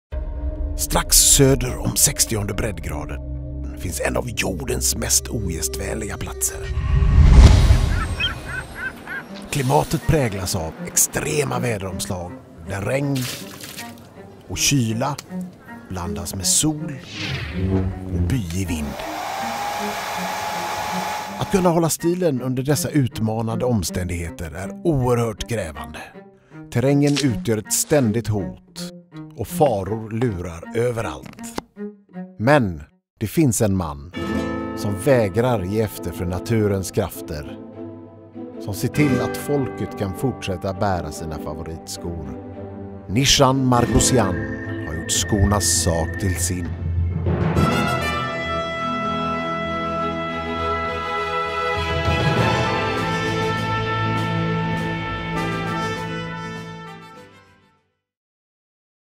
Vallgatans Skomakeri
Göteborg har ett extremt påfrestande klimat för skor. Att kunna hålla stilen under dessa hårda omständigheter är oerhört krävande. Utan Nishan hade många göteborgare gått en stilsäker död till mötes.

KAMPANJ AV:
Compañeros - Jennie Andersson, Rudolph-Hugo Rossouw, Maria Alexén, Anders Avehall